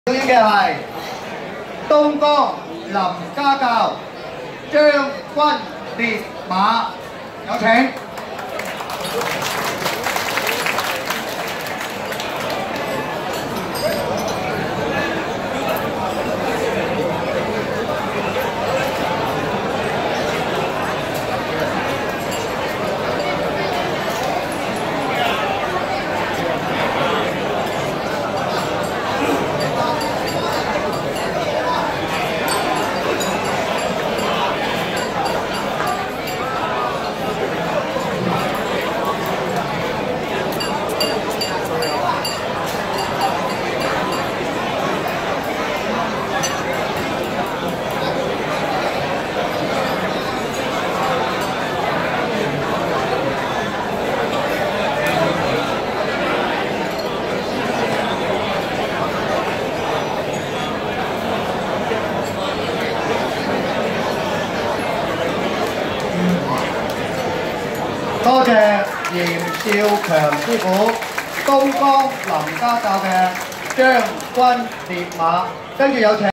龍形同心會八週年會慶-嚴少強師傅-東江林家教-將軍獵馬-2019/08/17
嚴少強師傅